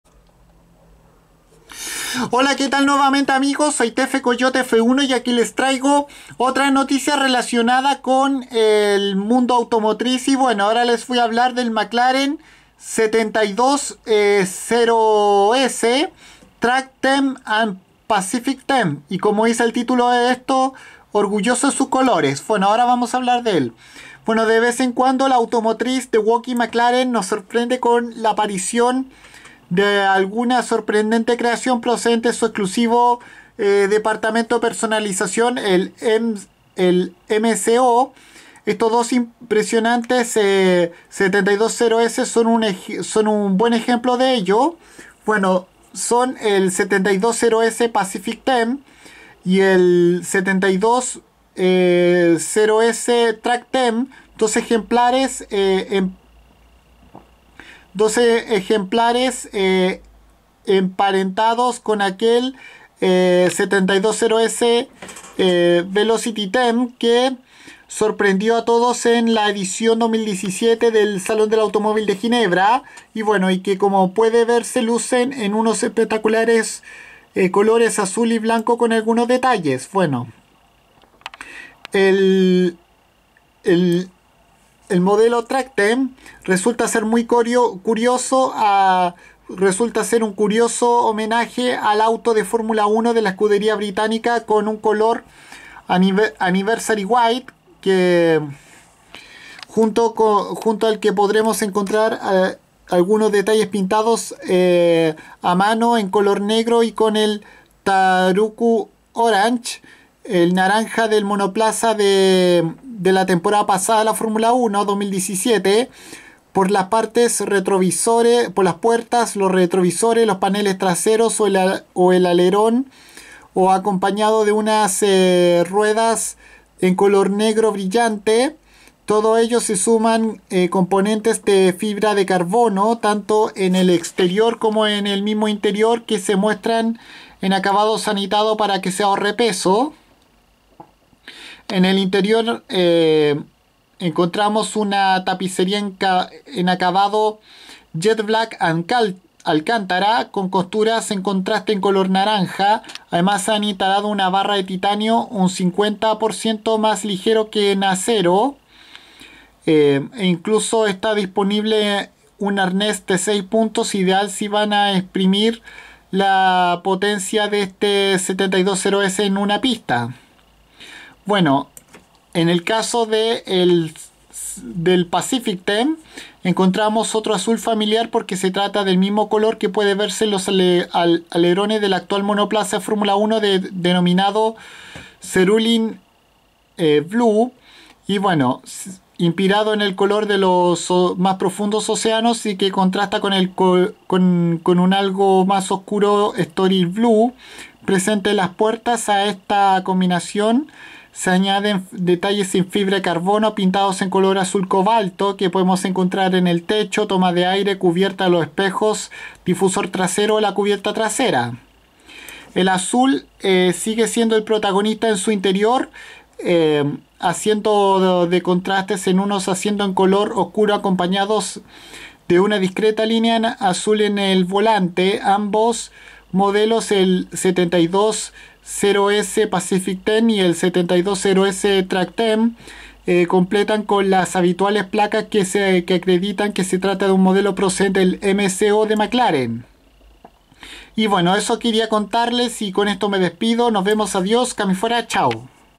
Información sobre los Mclaren 720S Pacific Theme y Track Theme
En este vídeo les enseño como son estas versiones especiales del 720S de la automotriz de Woking con colores inspirados en la F1 y realizados por MSO